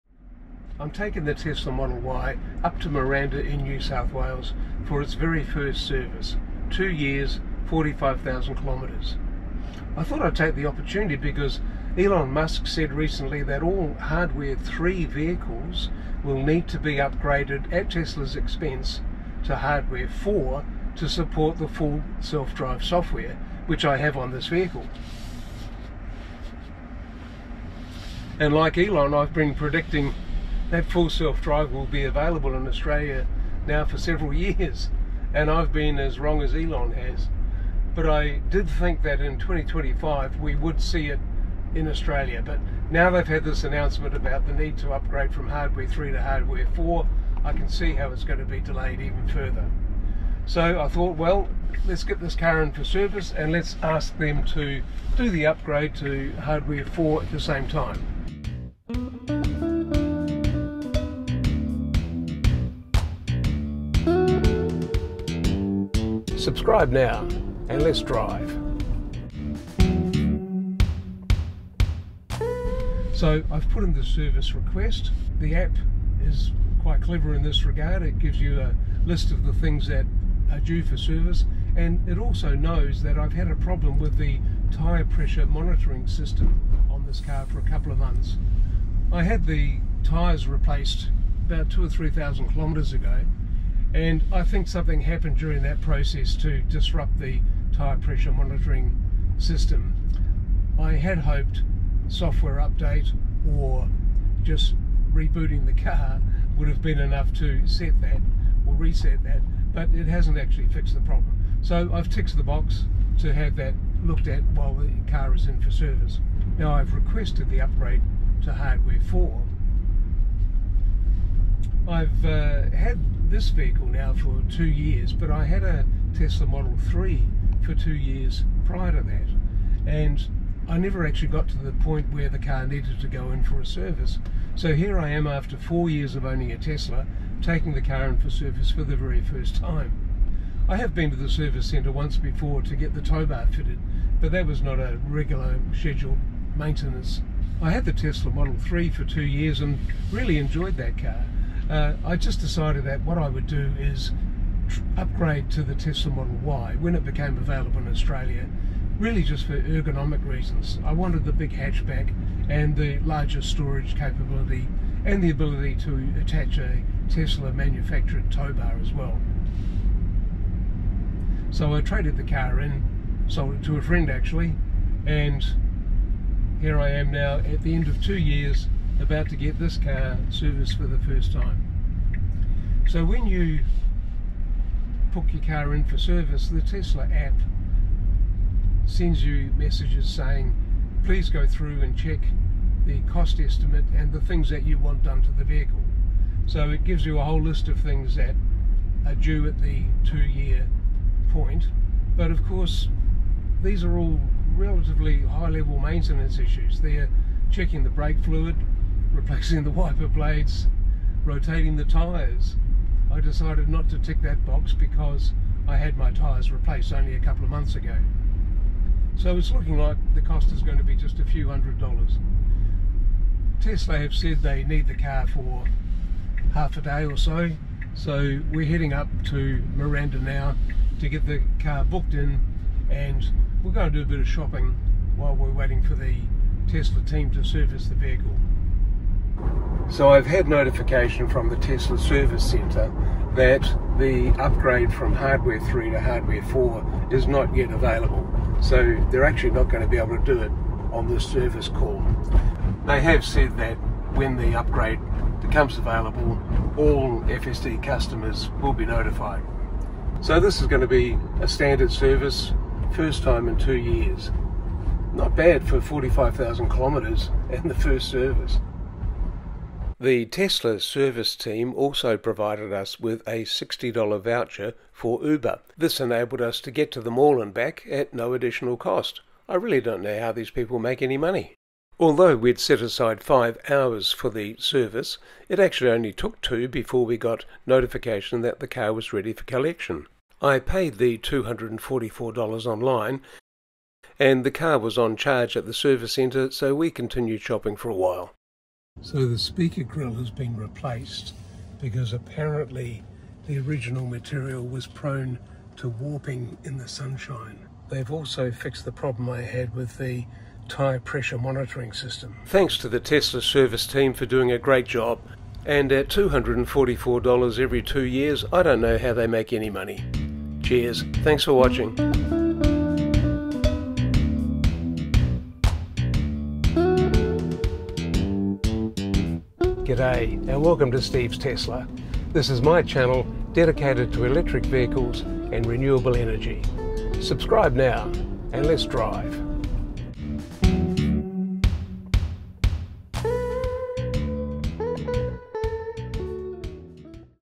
FSD upgrade Hardware 3 to HW4 at first service
My Tesla model Y is getting it's first service at 45,000km and 2 years. I've asked them to do the Hardware 3 to Hardware 4 upgrade at the same time.